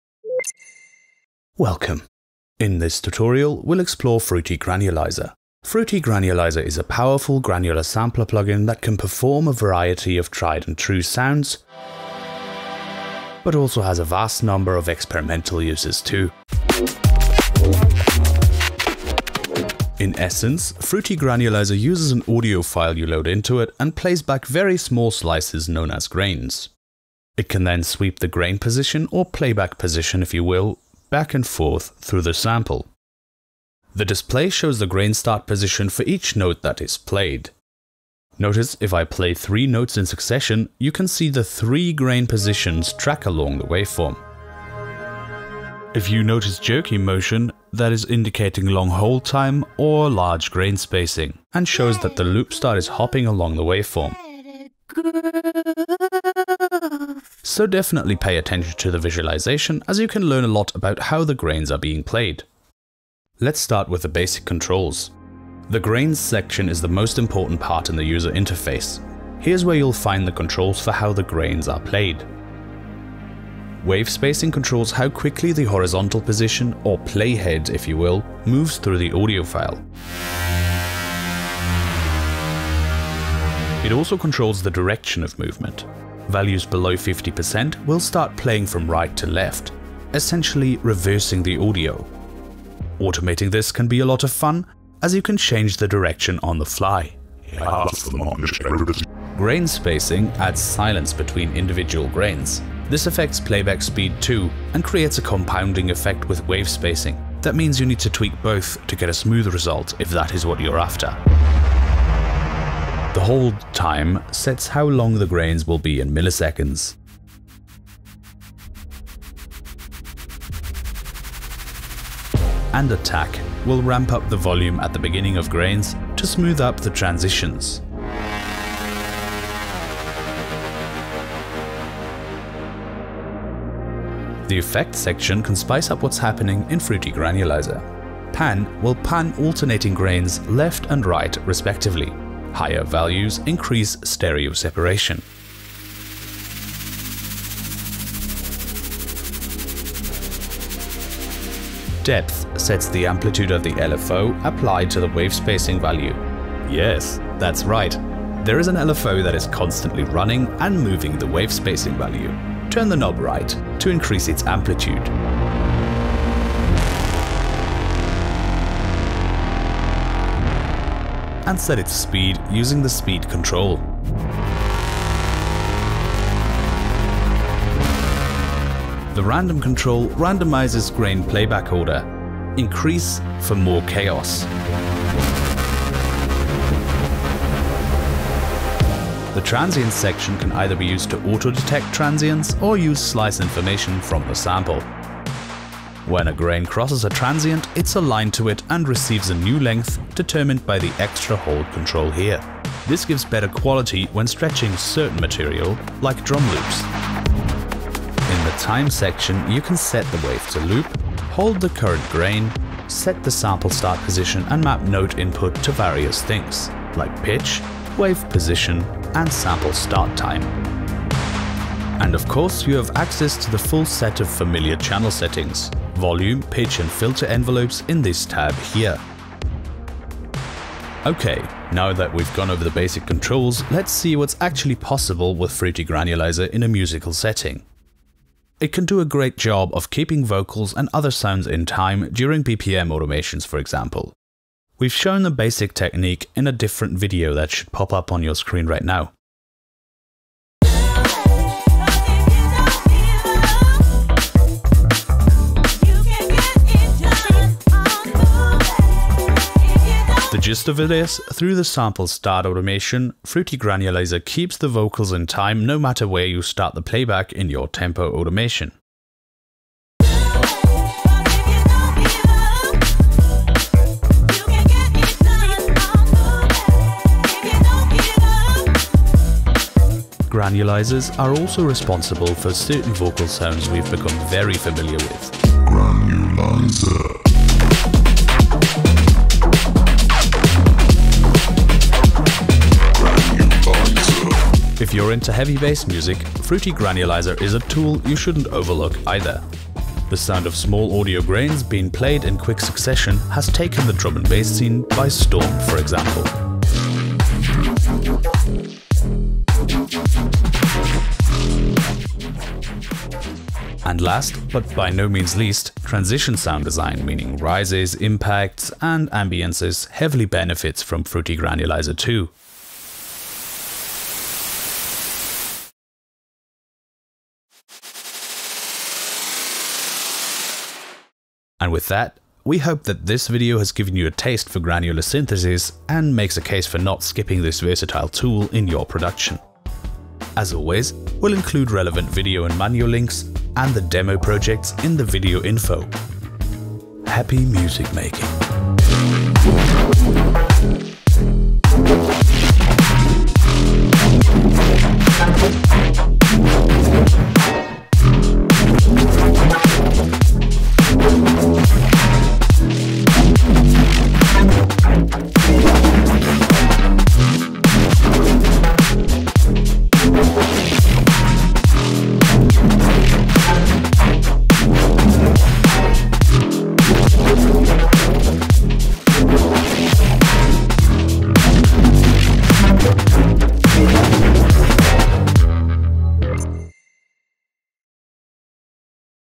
FL STUDIO | Fruity Granulizer
Video Index:
00:00 Introduction
00:19 How Granular Sampling Works
00:33 Fruity Granulizer | Wave Display
01:02 Basic Functions | Grains Panel
02:13 Basic Functions | Effects Panel
03:08 Basic Functions | Transients Panel
03:27 Basic Functions | Time Panel
03:51 Sound Examples
04:39 Cool Granulizer
05:47 Outro

Image-Line Software